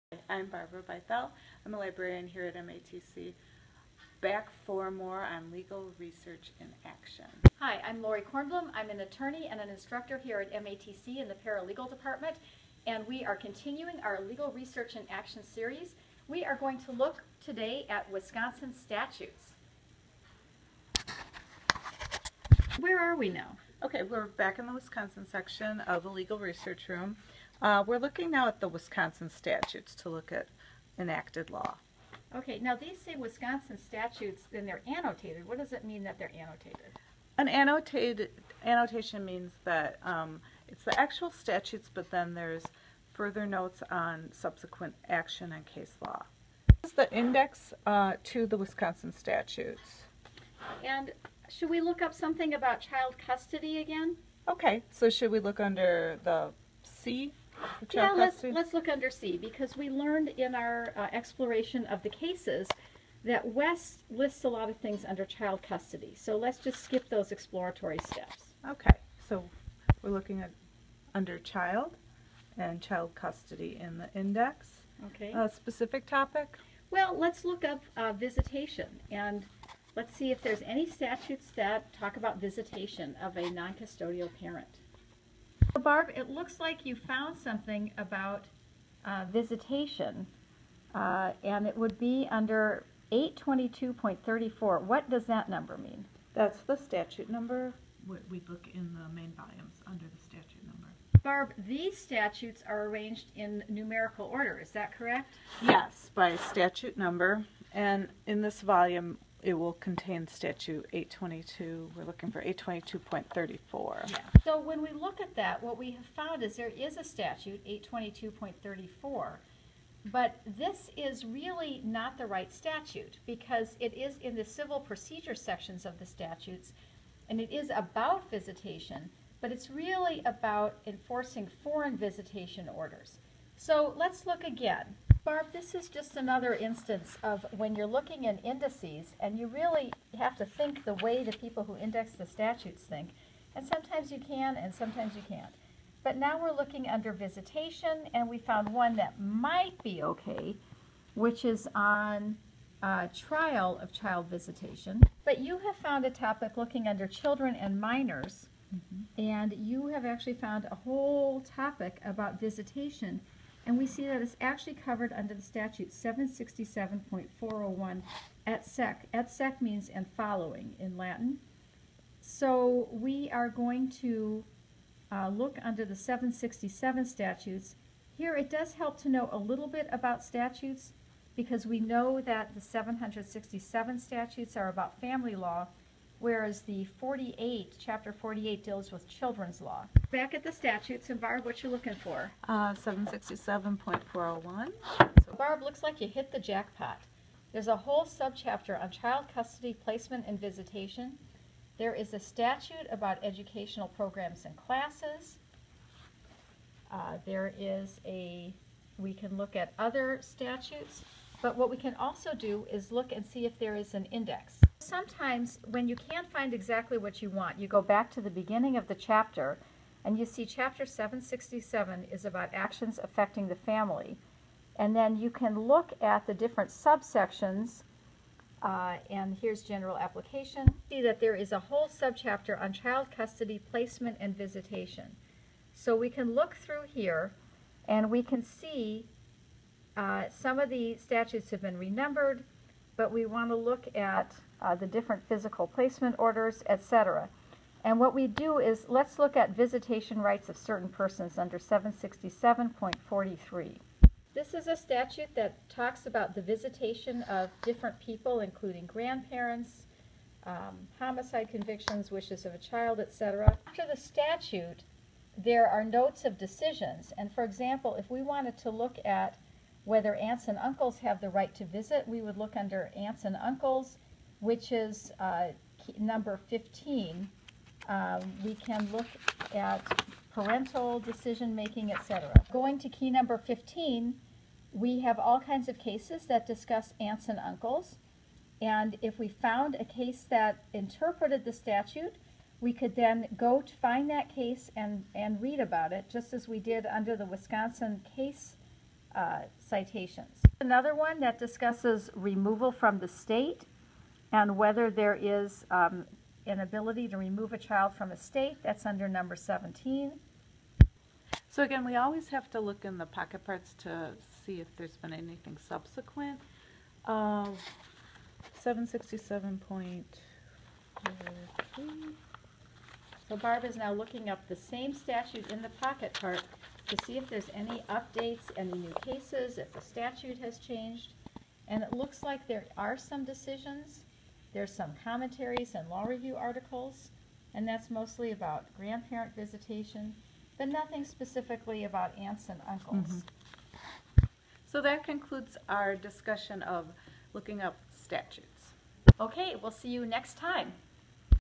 Legal Research: Researching Wisconsin Statutes-MATC Legal Research in Action
Librarian Barbara Bythell and Attorney-Instructor Lori Kornblum show you how to research Wisconsin Statutes.